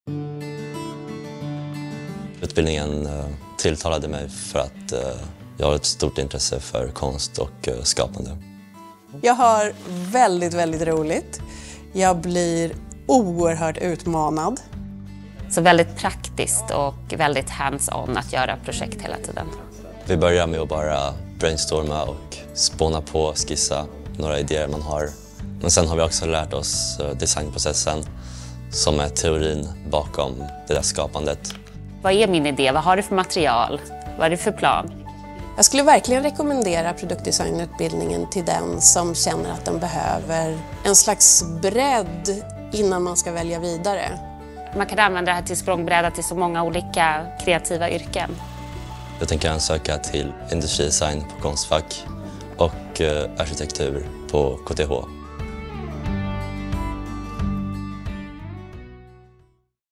Produktdesign på Arkitekturskolan STHLM
Vill du förändra världen med design? På utbildningen Produktdesign lär du dig att visualisera dina idéer och arbeta fram förslag på produkter som ger oss en hållbarare och bättre framtid. Utbildningen är studiemedelsberättigande och förbereder dig för studier på högskola.